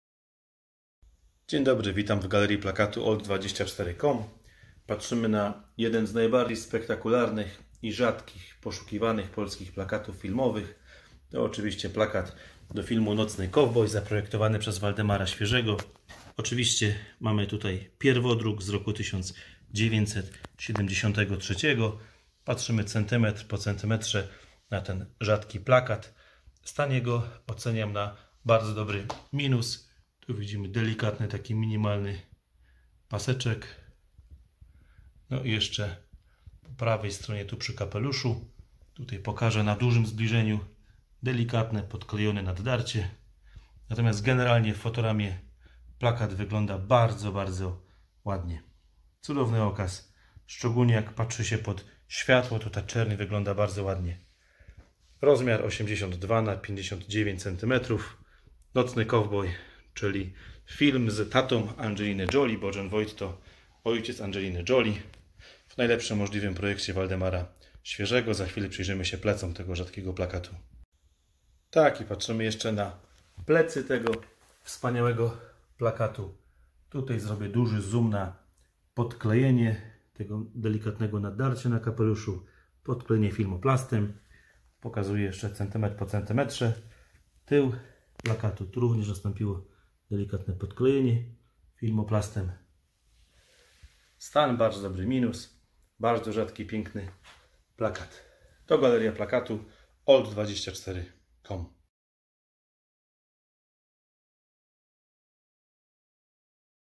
Nocny kowboj Waldemar Świerzy Polish poster to Midnight cowboy plakat filmowy
FILM PRZEDSTAWIA JEDEN Z NAJBARDZIEJ POSZUKIWANYCH I ZNANYCH POLSKICH PLAKATÓW NA ŚWIECIE.

Oryginalny polski plakat filmowy.
Pierwodruk.

Stan bardzo dobry-. Delikatne naddarcia podklejone taśmą.
Zobacz na szczegółowe fotografie plakatu.

THE FILM PRESENTS ONE OF THE MOST WANTED AFTER AND FAMOUS POLISH POSTERS IN THE WORLD.

Original polish movie poster.
First edition.

Condition: Very good-. Delicate tears sealed with tape.
See detailed photographs of the poster.